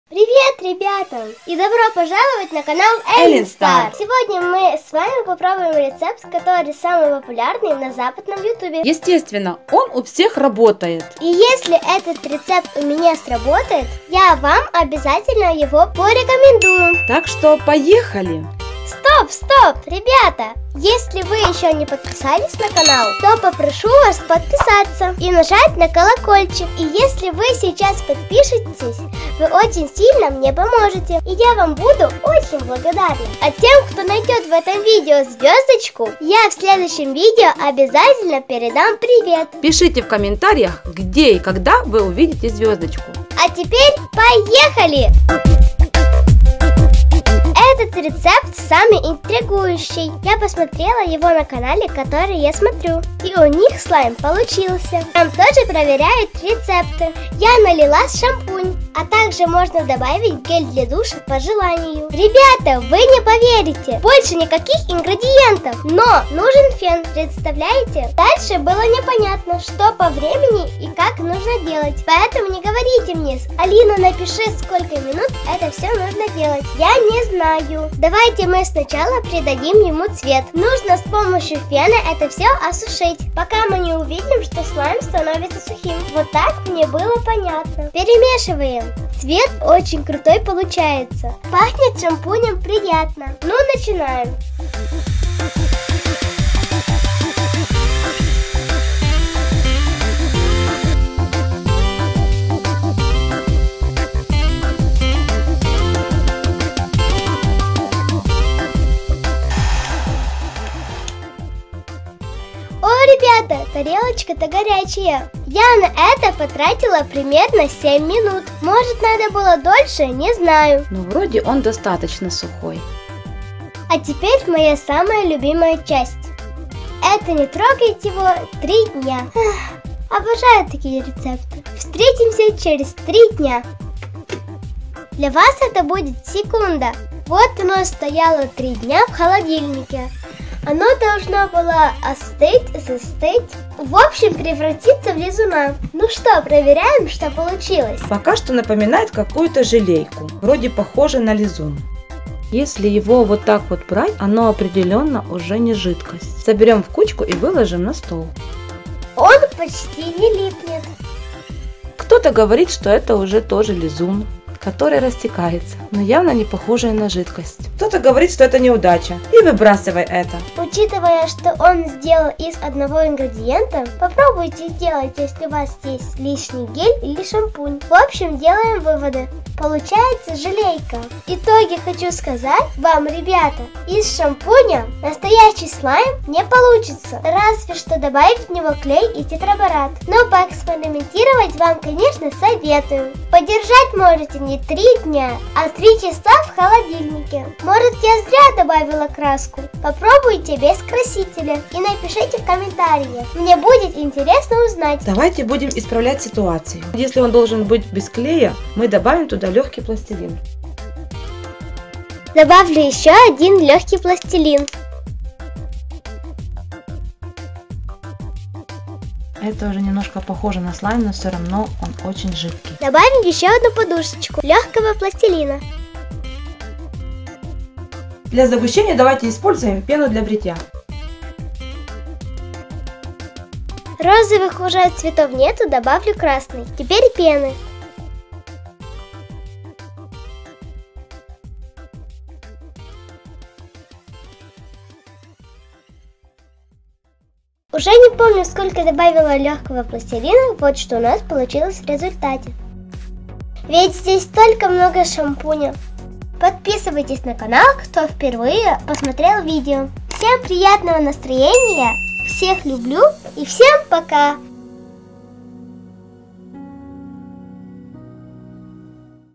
Лизун без клея и тетрабората натрия Слайм из шампуня | Elin Star
Лизун без клея и тетрабората натрия  Слайм из шампуня на канале Elin Star
КАК СДЕЛАТЬ ЛИЗУНА ИЗ ШАМПУНЯ БЕЗ КЛЕЯ И ТЕТРАБОРАТА НАТРИЯ 😉
В этом видео мы попробывали сделать слайм из шампуня, лизуна без клея и тетрабората натрия всего из одного ингредиента!

СПАСИБО!
🌟Если видео понравилось поставьте палец вверх 👍 СПАСИБО!!!😊💖